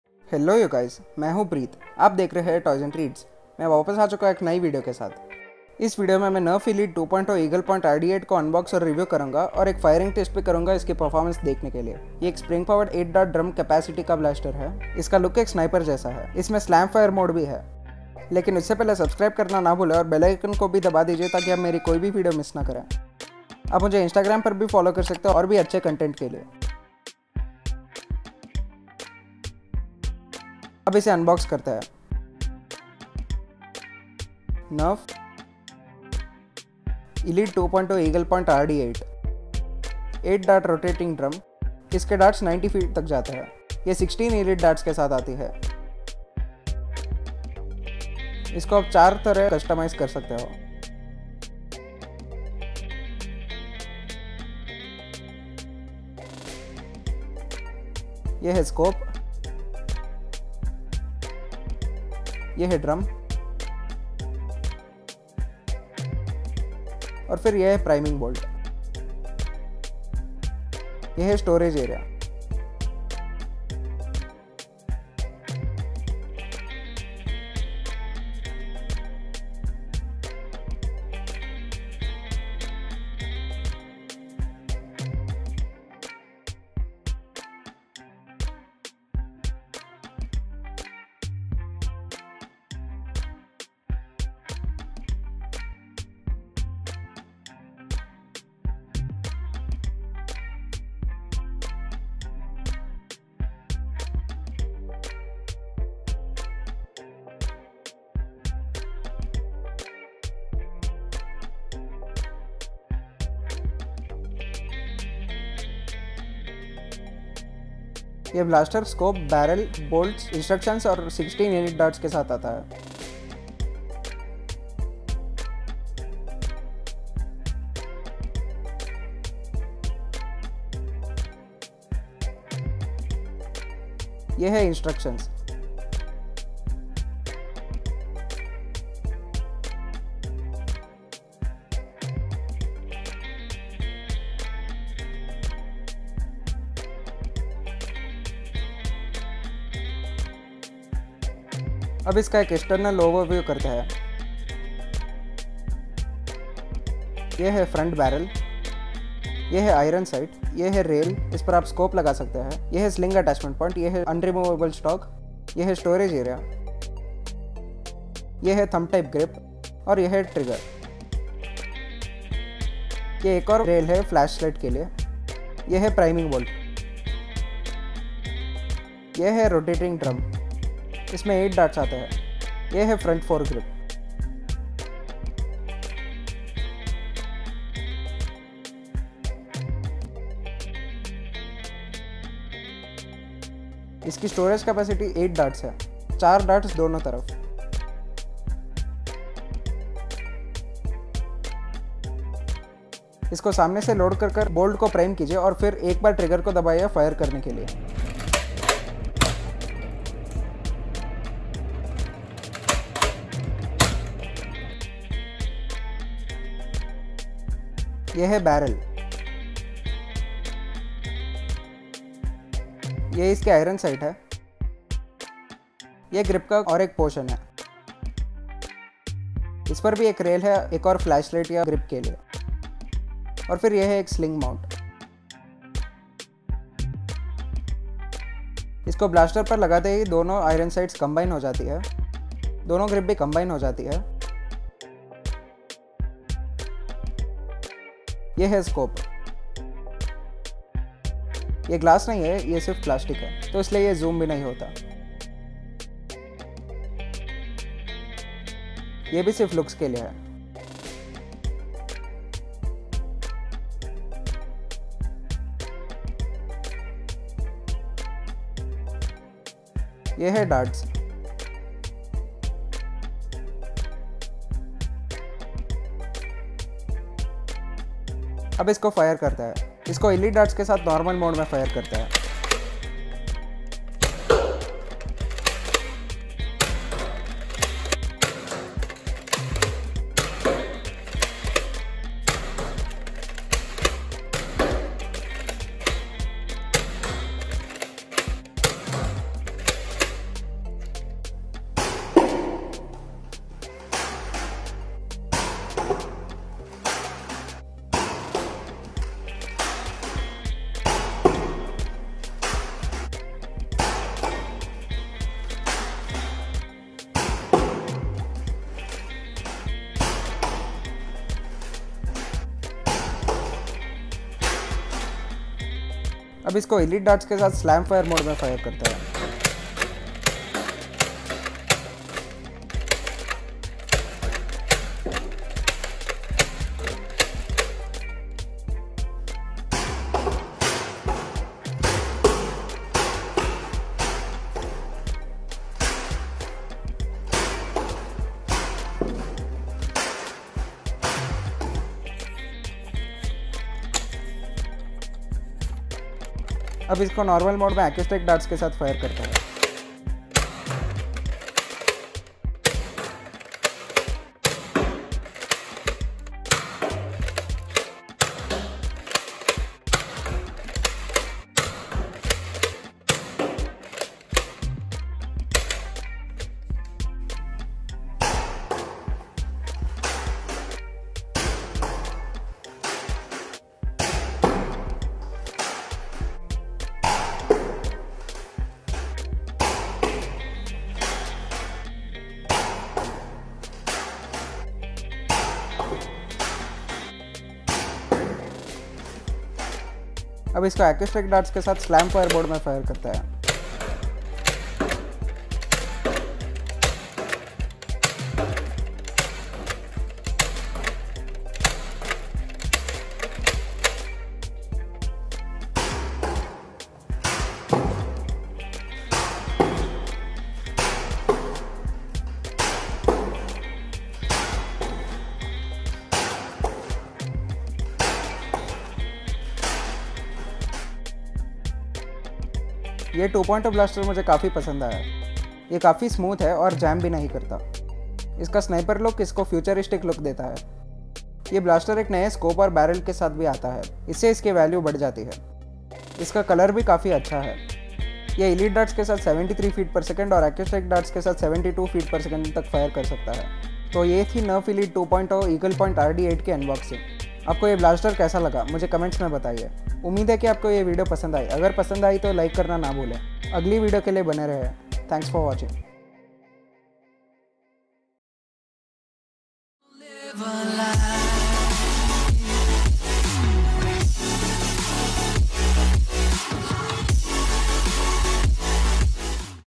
A Good Elite 2.0 Blaster!? | Hindi | Nerf Elite 2.0 Eaglepoint RD-8 |Unboxing|Review|Firing Test
Nerf Elite 2.0 Eaglepoint RD-8 | Unboxing | Review | Firing Test | Hindi |